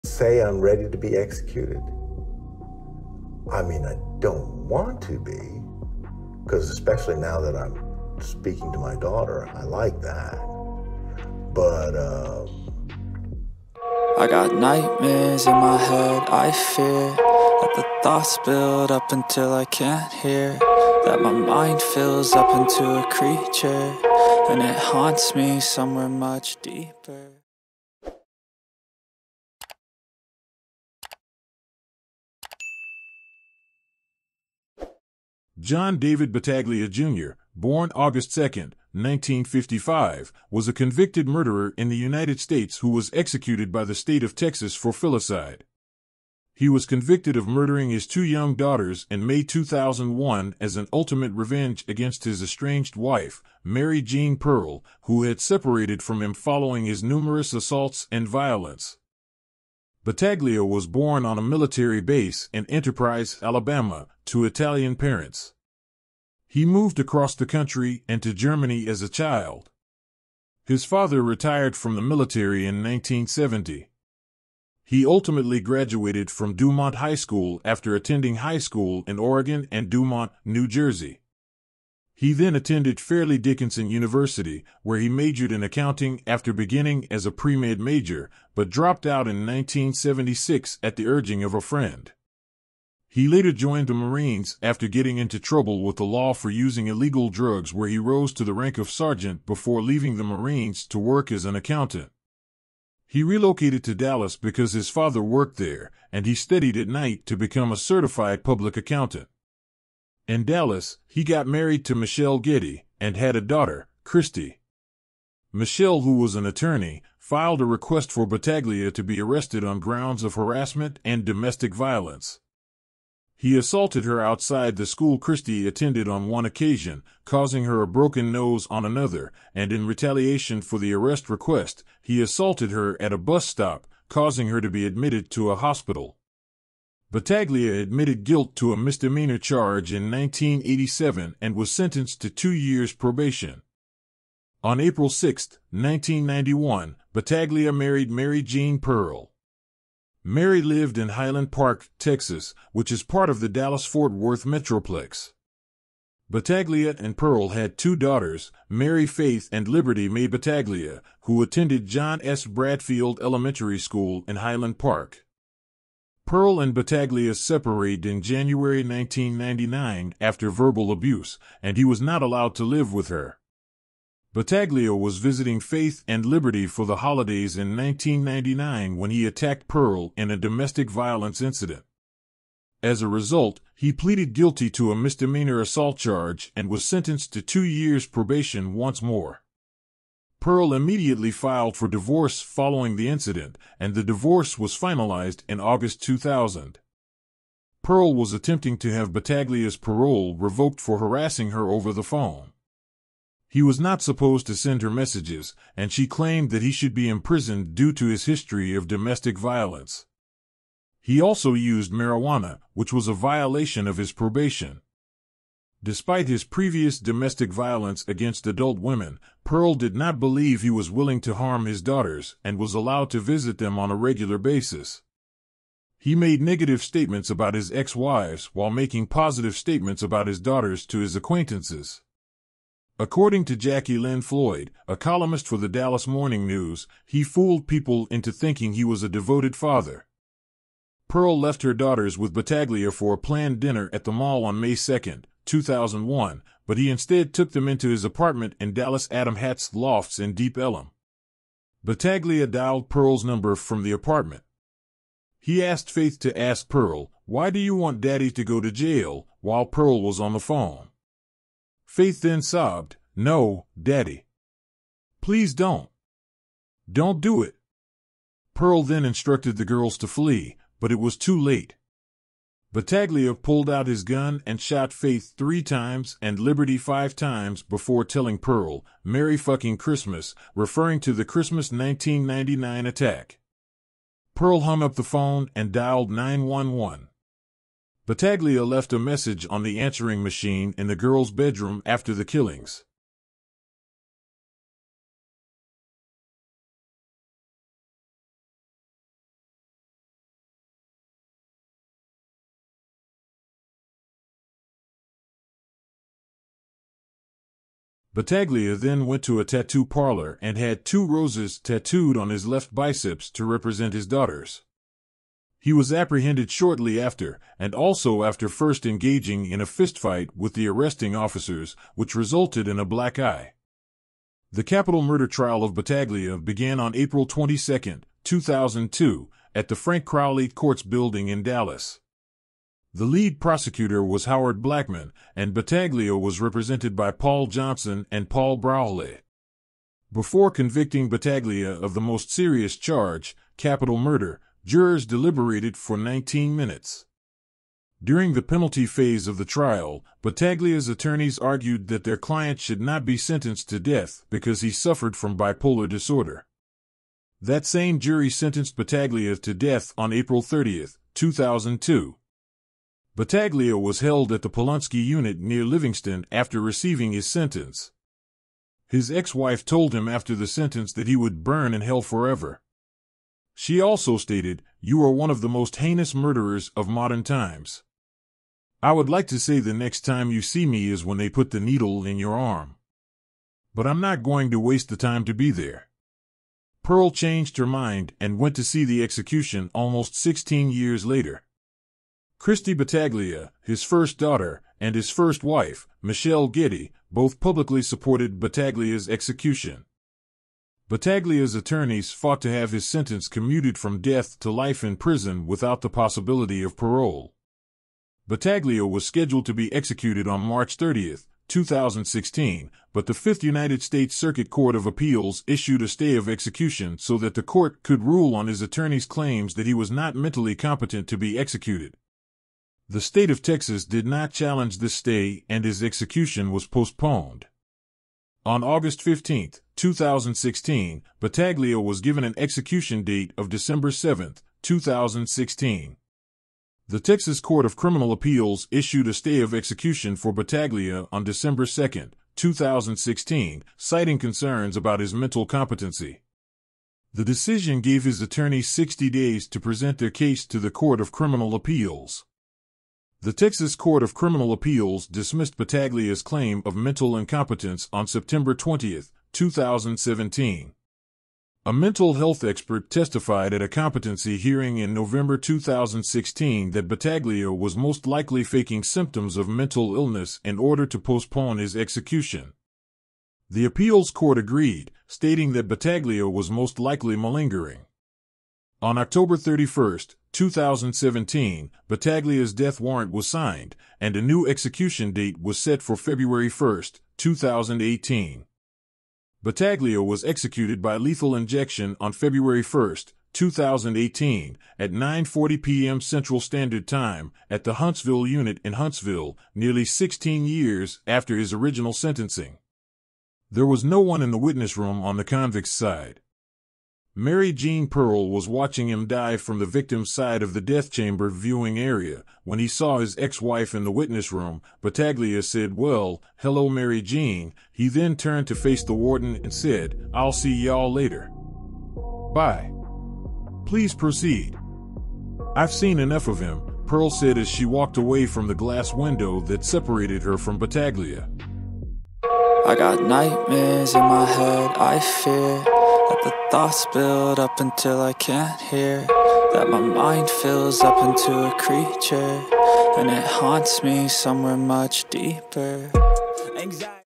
THE STORY OF JOHN BATTAGLIA: THE FATHER WHO MURDERED HIS OWN KIDS
John David Battaglia Jr. was an American convicted murderer who was executed by the state of Texas for filicide. He was convicted of killing his two young daughters in May 2001 in an act of "ultimate revenge" against his estranged wife, Mary Jeane Pearle, who had separated from him after his numerous instances of assault and violence. Battaglia was executed for the murders on February 1, 2018.